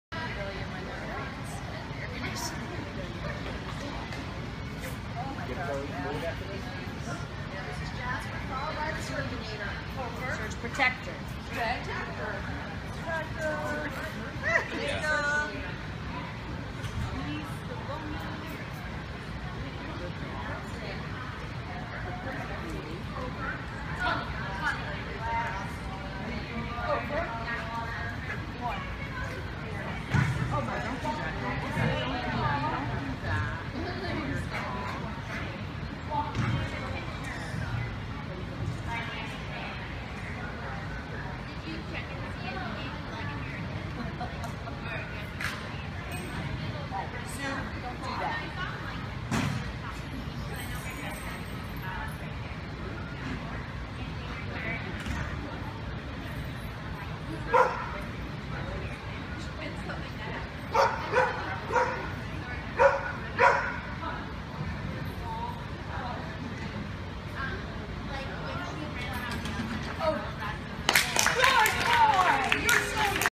UKI Agility 20150719
Decided to give this one a real shot and did not run NFC. Still making a lot of mistakes, but still having a lot of fun and getting better little, tiny baby steps at a time.